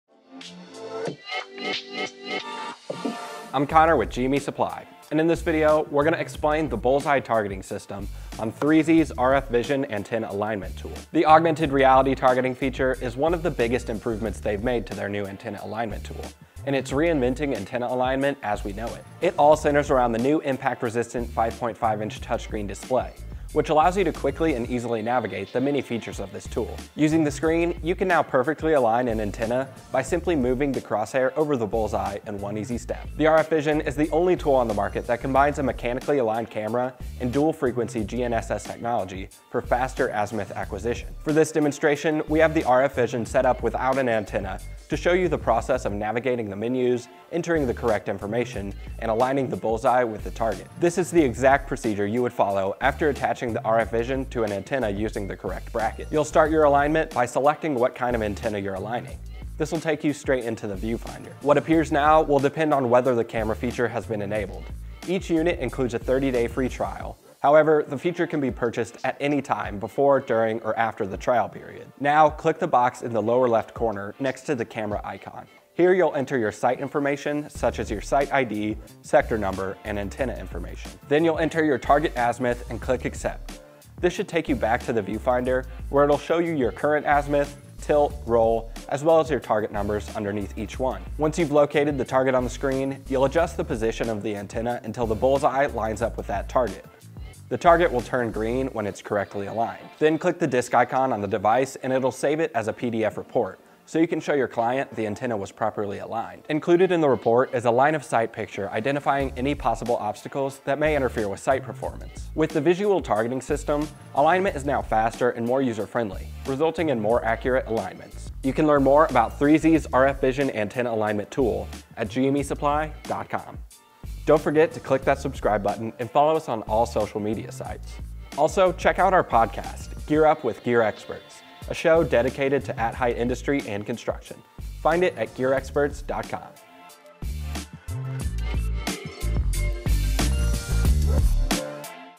RF Vision - Bullseye Targeting System
The mechanically aligned camera paired with augmented reality crosshairs are redefining antenna alignment. RF Vision

Achieving proper alignment is now easier and more accurate than ever with the RF Vision from 3Z. Learn more about how to use the bullseye targeting feature in this video.